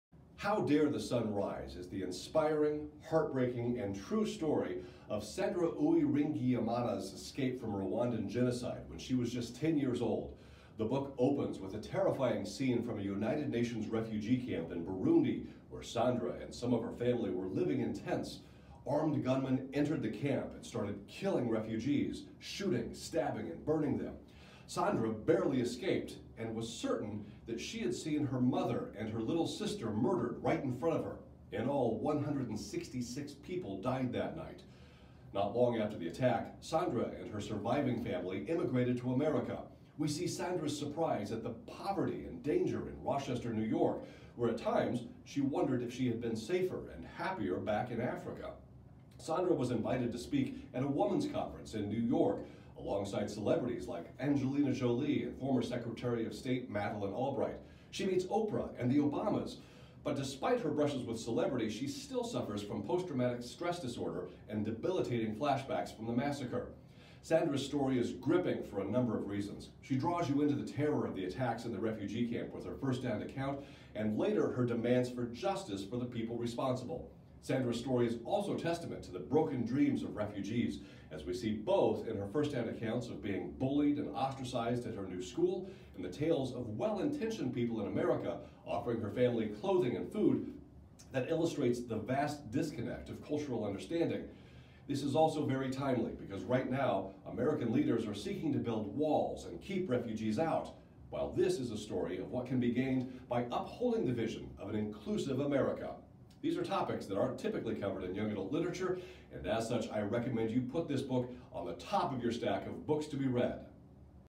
How Dare The Sun Rise by Sandra Uwiringiyimana 60 second book talk
How Dare The Sun Rise is the true story of Sandra Uwiringiyimana's escape from the Rwandan massacre of her childhood, and the disappointing immigration to the United States, a land of her hopes and dreams. Similar to I Am Malala by Malala Yousefzai.